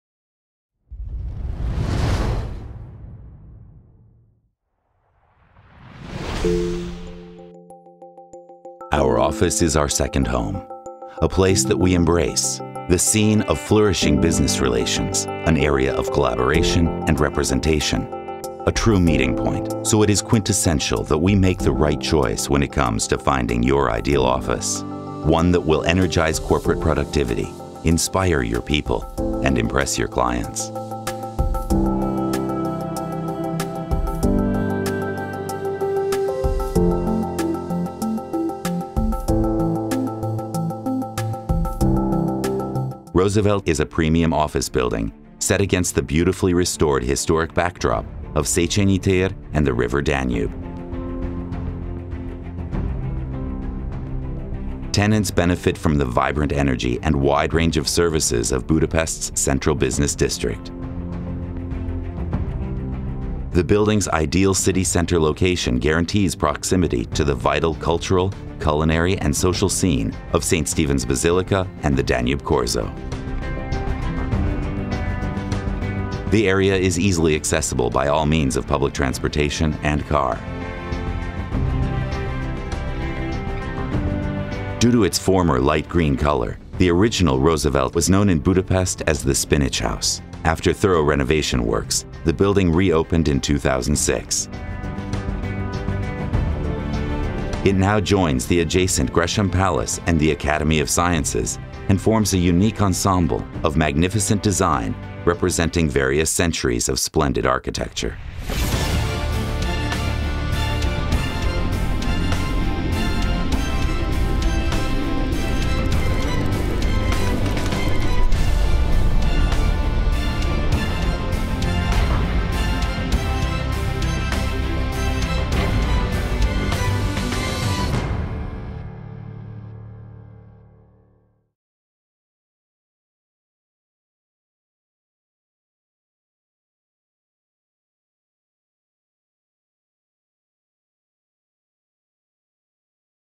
Roosevelt Office Building, Budapest
Roosevelt is a category ‘A’ premium office building set against the beautifully restored historic backdrop of Széchenyi Square and the river Danube. Tenants benefit from the vibrant energy and wide range of services of Budapest’s Central Business District.

The office building excels with its prime location, state-of-the-art design, spacious and inviting common areas, efficient floor plates, great quality built-in materials, abundant natural light and cost-effective service charges. The building’s ideal city center location guarantees proximity to the vital cultural, culinary and social scene of Saint Stephen’s Basilica and the Danube corso.

Due to its former light green color, the original Roosevelt (completed in 1980) was known in Budapest as the ‘Spinach House’. After thorough renovation works, the building re-opened in 2006. It now joins the adjacent Gresham Palace and the Academy of Sciences and forms a unique ensemble of magnificent architecture representing various centuries. The area is easily accessible by all means of public transportation (metro, bus, tram, boat) and car.